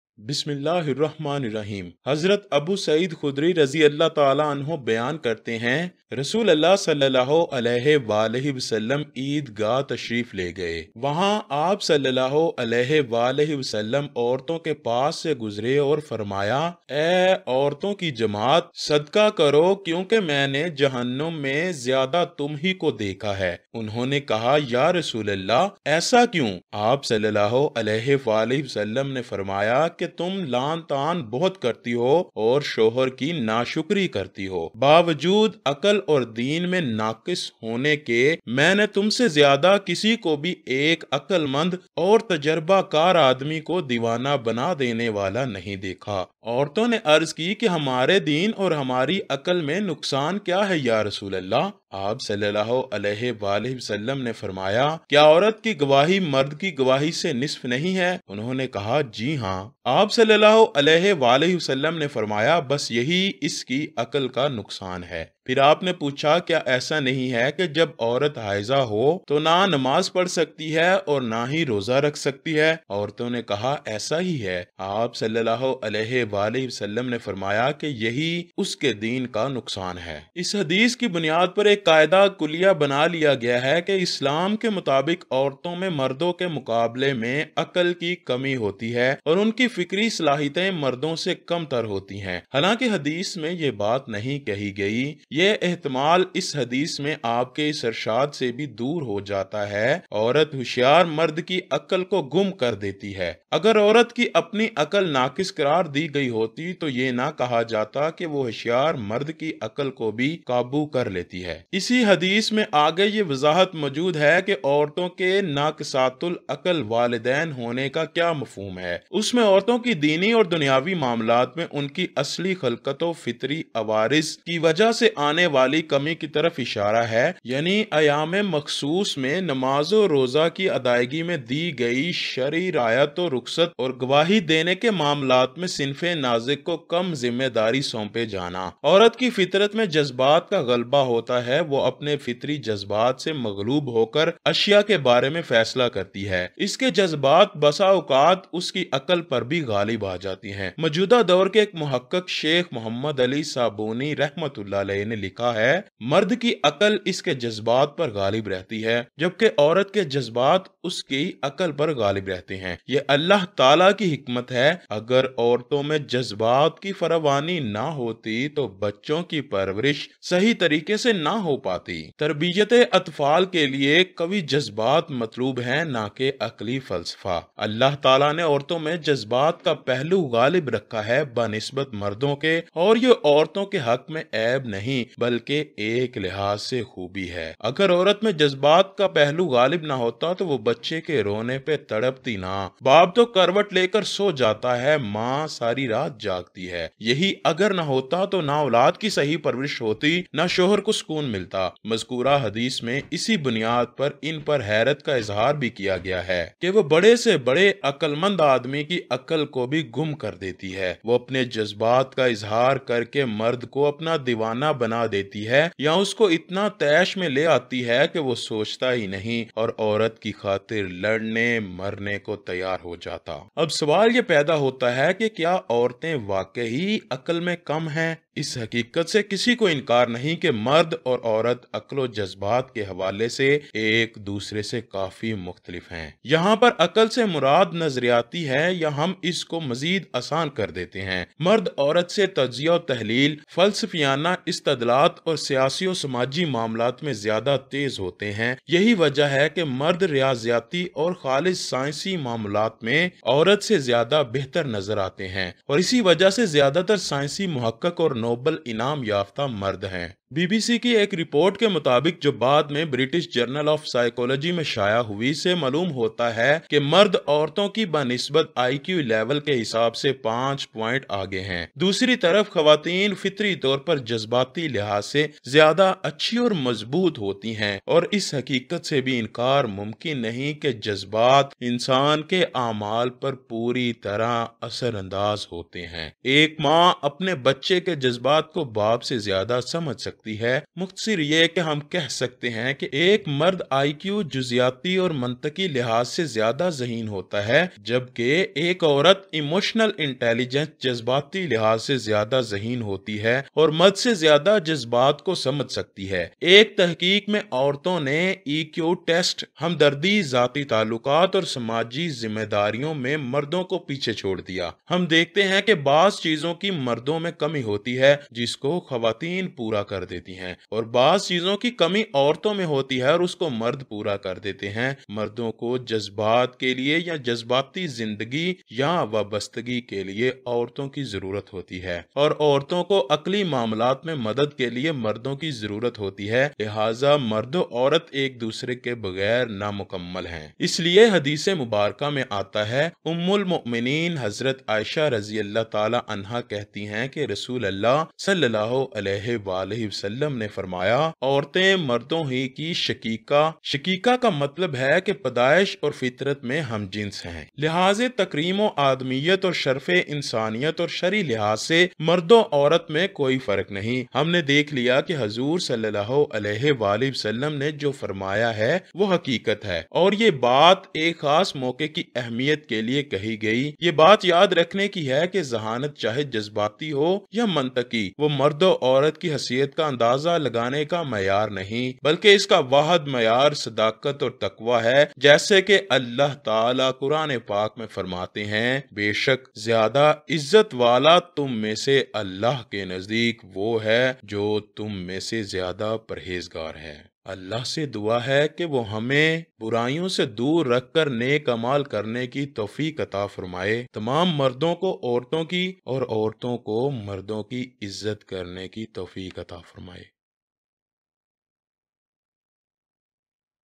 Aurat Kam Aqal Hoti hai ? Hadees || Islamic Story | Sabaq Amoz Kahani in Urdu | Urdu Islamic Stories
Label : Kashif Voice
Editor : M.Ali